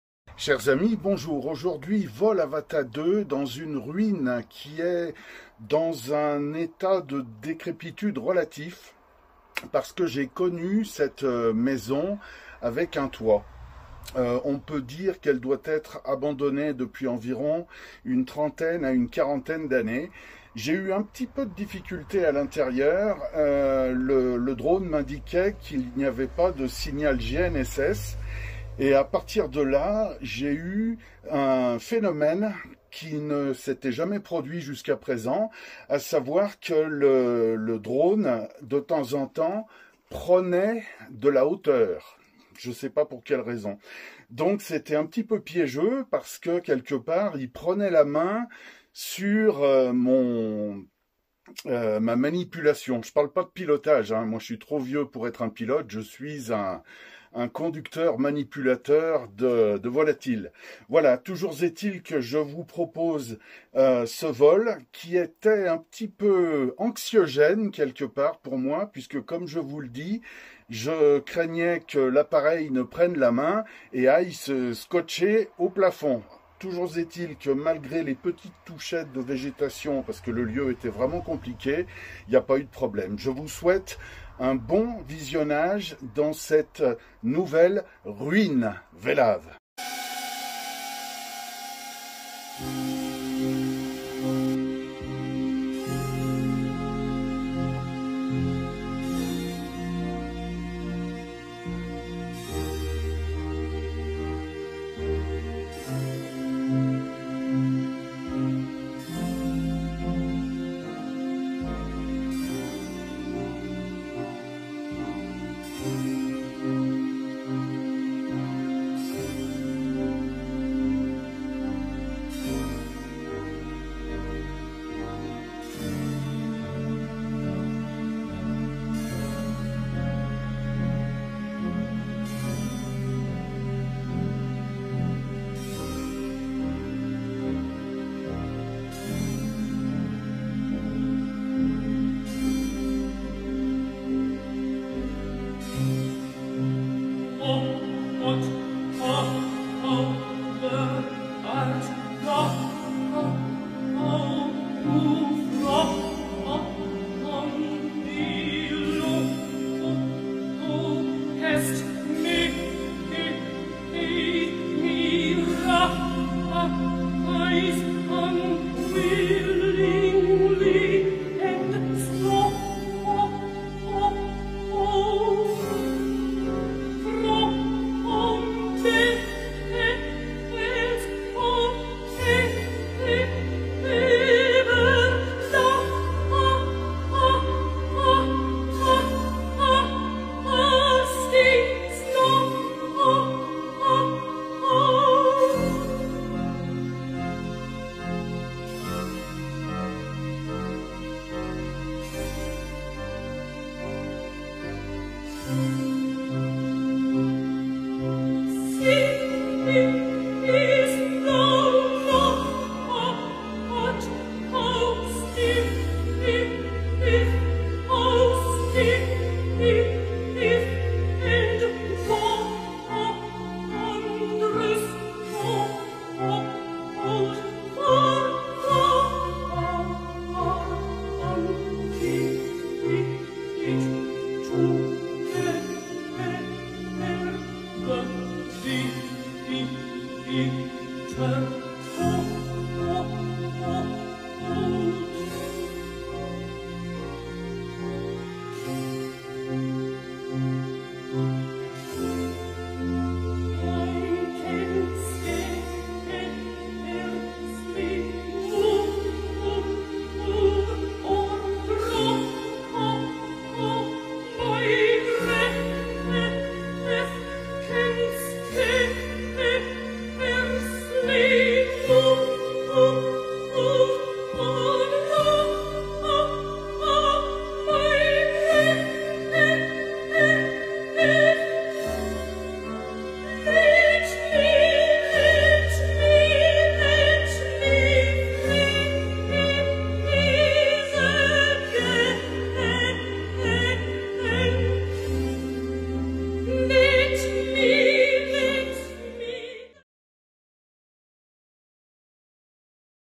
DJI Avata 2 : exploration d'une ruine vellave abandonnée depuis une trentaine d'années
DJI Avata 2 FPV drone flight
Happy daily flight
Exploration d'une ruine vellave abandonnée depuis une trentaine d'années. Ferme, habitation et dépendances
Problèmes de réception GNSS et basse luminosité : l'un OU l'autre ou l'un ET l'autre de ces facteurs ayant entraîné des prises de hauteur subites par le drone, indépendantes de ma volonté
Environnement ultra complexe : énormément d'obstacles cependant évités grâce à la liaison O4 des DJI goggles 3
Filmé sur Realme GT6T et DJI Avata 2
Musique ajoutée sur éditeur DJI Fly
Images coupées et assemblées sur éditeur DJI Fly